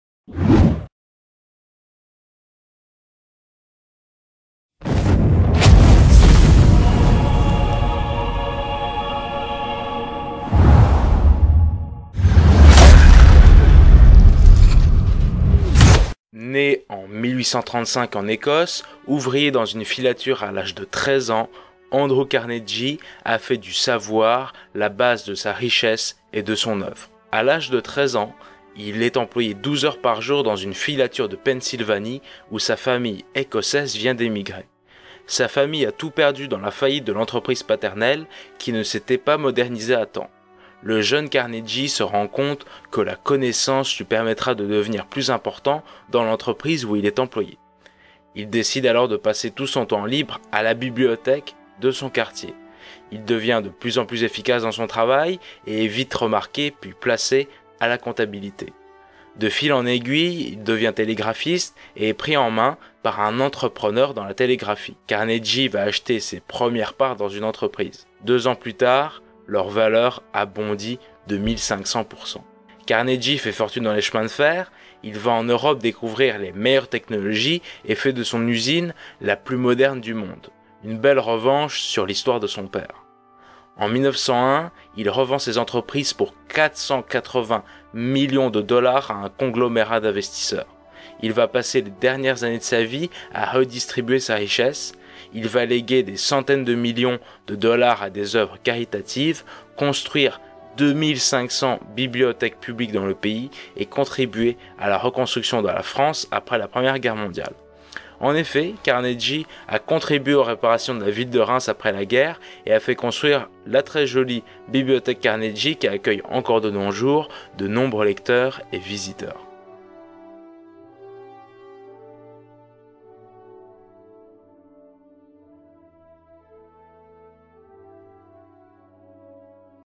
5 faits méconnus sur Carnegie
5 faits méconnus sur Carnegie est le sujet de la présente vidéo. Découvrons ensemble quelques faits méconnus sur l’un des hommes les plus riches de l’Histoire, Andrew Carnegie, qui fut aussi l’un des prédécesseurs d’une vision philanthropique du capitalisme.
Né en 1835 en Ecosse, ouvrier dans une filature à l’âge de 13 ans, Andrew Carnegie a fait du savoir la base de sa richesse et de son œuvre, faisant de lui un précursseur du mouvement philanthrope, qui voyait de grandes fortunes participer par leur générosité à l'amélioration de la vie en communauté.

1.Le jeune ouvrier
2.La bibliothèque
3.Les chemins de fer
4.La fortune colossale
5.Le philanthrope